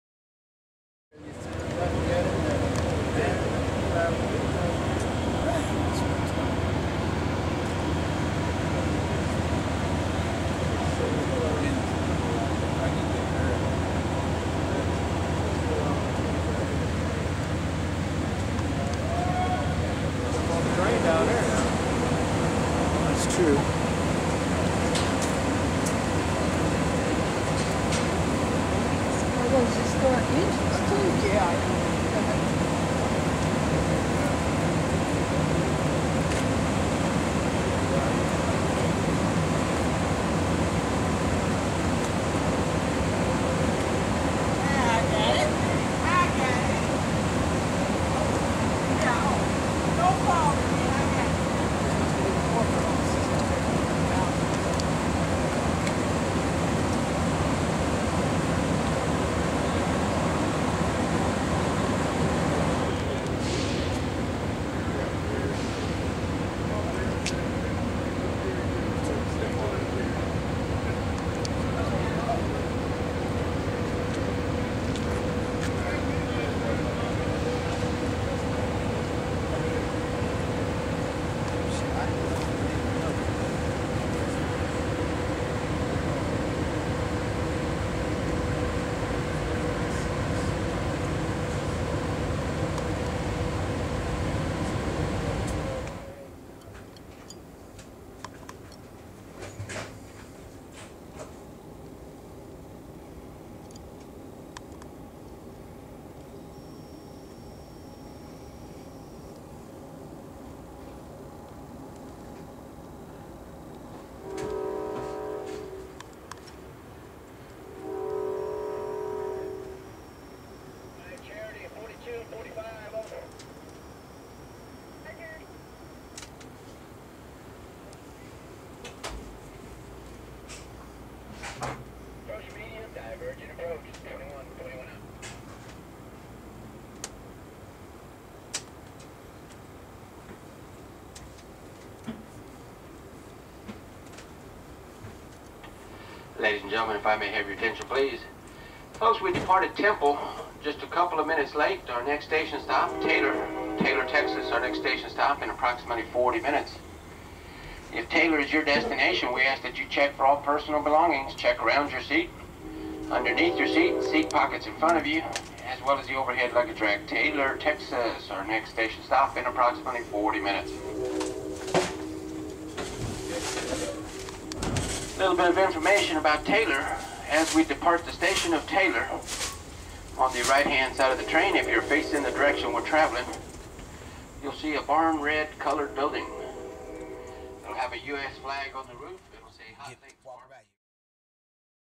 Amtrak Texas Eagle #21 at Temple, TX: 10-26-2010
Amtrak Ride 2010: Texas Eagle, Extended Scene - As the Texas Eagle sits in Temple, TX, we get our first look at the Temple Railroad and Heritage Museum.